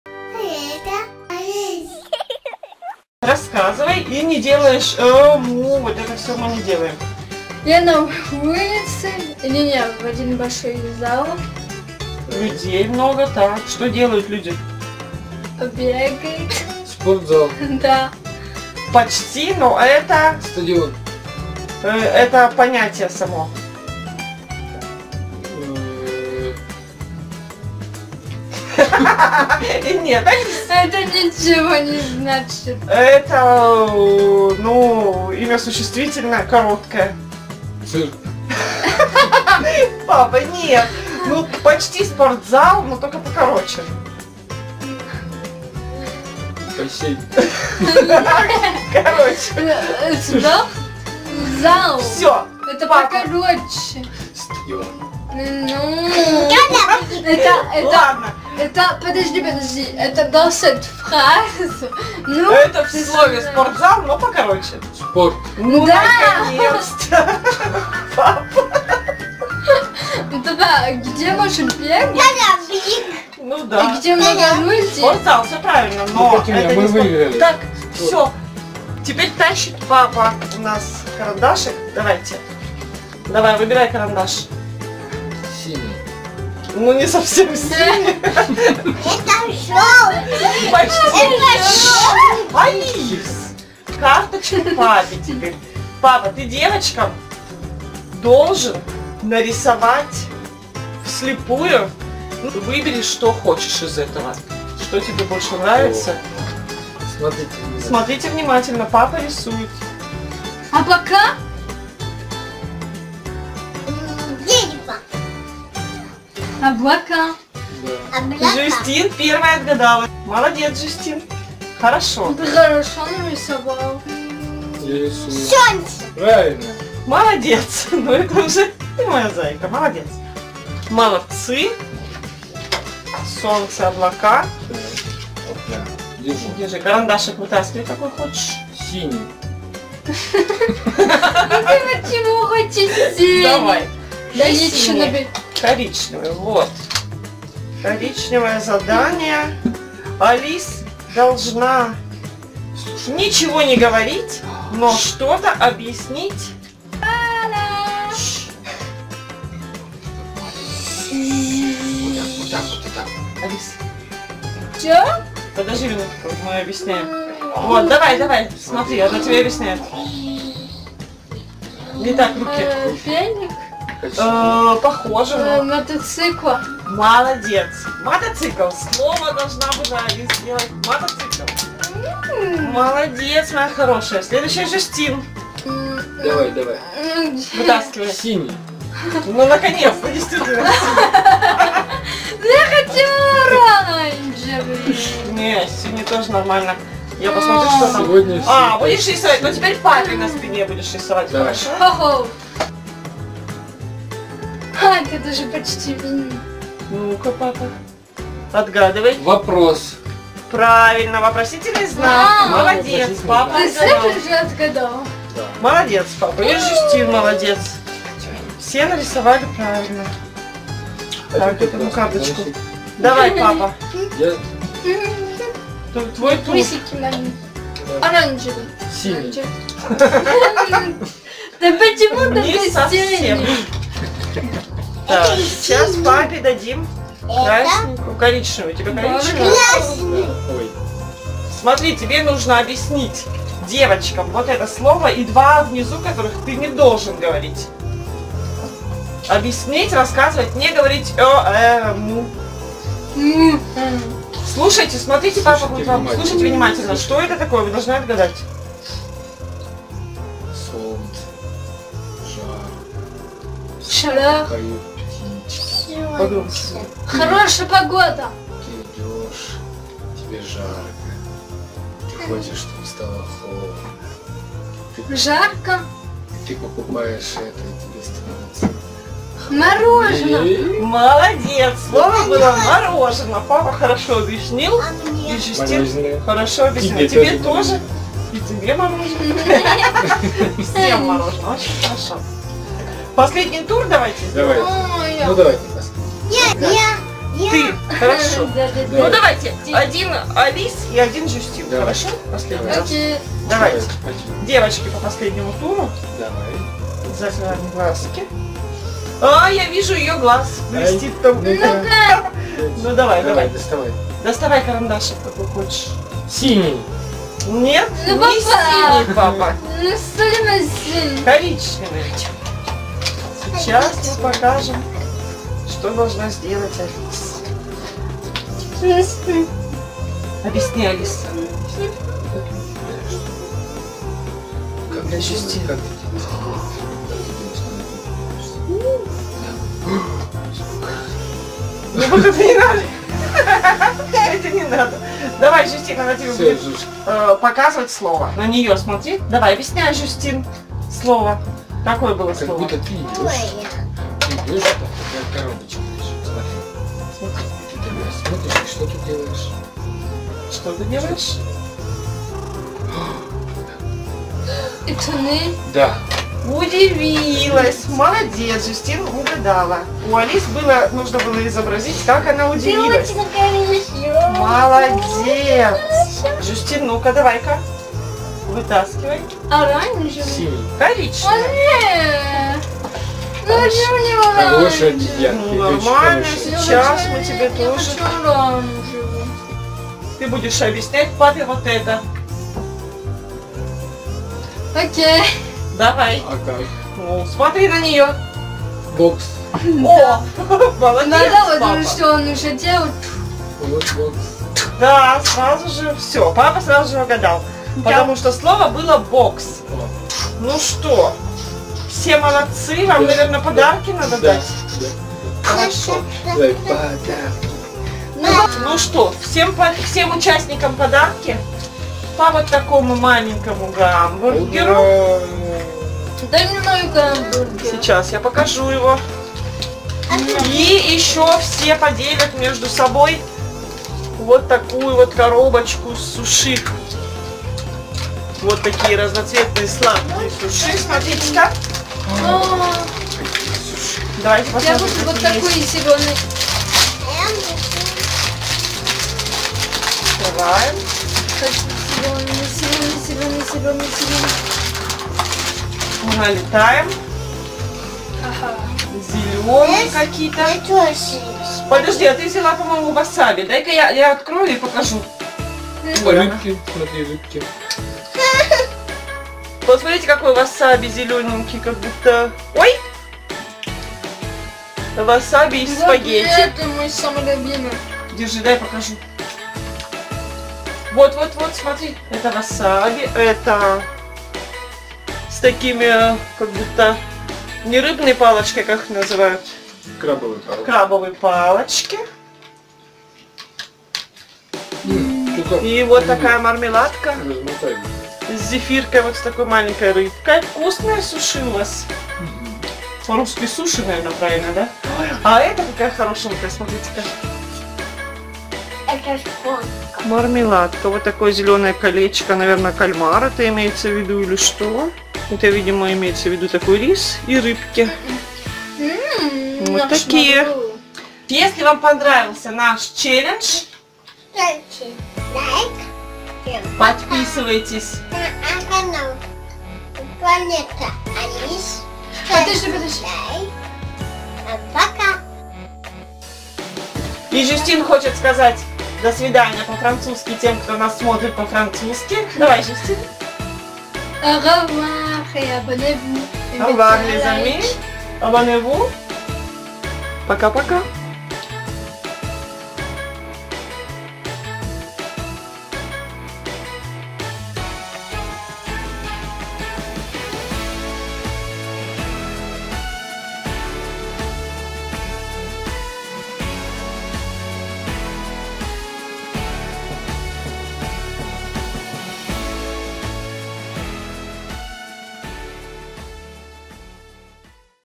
ШЕПОТ ЮТУБЕРА Челлендж Ничего не вижу ничего не слышу ничего не скажу // 2 серия
Алис вместе с папой и сестрой Жюстин устраивают супер-челлендж "Шепот ютубера" или Не вижу Не слышу Не говорю.  2 серия. Рисуем "вслепую", угадываем слова, рисуем на спине...Всем участникам - сладке призы: мини-гамбургер и сладкие суши:)